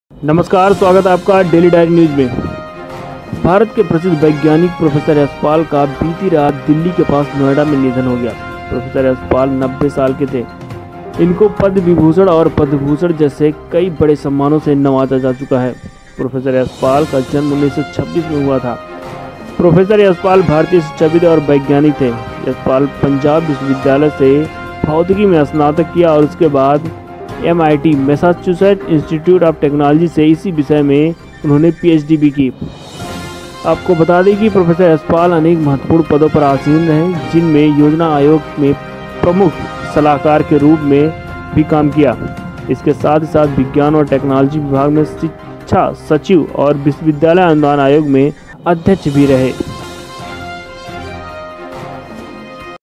पद्म भूषण वैज्ञानिक Professor Yashpal का 90 साल की उम्र में निधन
Globally renowned physicist, academic and higher education reformer Professor Yash Pal passed away in Noida on Tuesday. He was 90. The highly-regarded academic began his career at Mumbai's Tata Institute of Fundamental Research. He was a member of the 'cosmic rays group'. He then attended the Massachusetts Institute of Technology for his PhD, after which he returned to the Tata Institute, where he remained until 1983. Yash Pal was appointed by the government to several school and higher education reform committees. When the National Council of Educational Research and Training began work on the National Curriculum Framework, Yash Pal was roped in to chair its steering committee. In 2009, the human resources development ministry set up a committee on higher education - that came to be called the Yash Pal Committee - which Yash Pal chaired, to look into reforming higher education in the country.